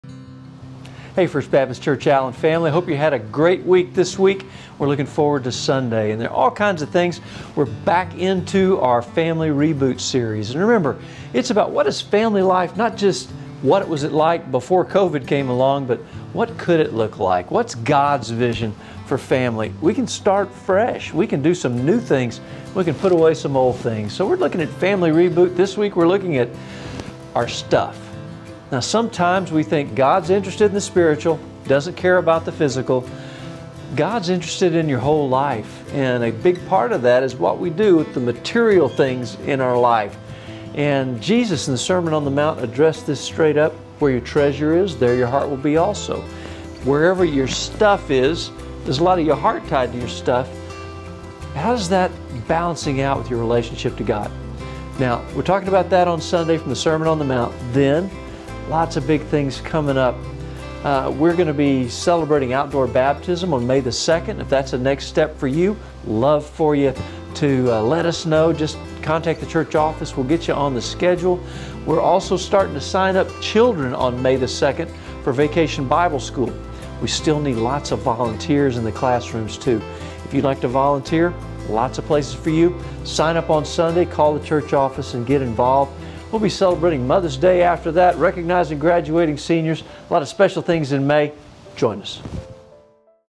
Your First Look at the Week of April 25th
In many ways, life is beginning to return to “normal”. In our Family Reboot series, we are looking at habits and routines that we should leave behind. This is a great season to set new patterns and priorities for our families and looking at “our stuff” may be a good place to start. Jesus told 38 parables, and one-third of them dealt with possessions. How we deal with “stuff” is a reflection of our hearts and spiritual health.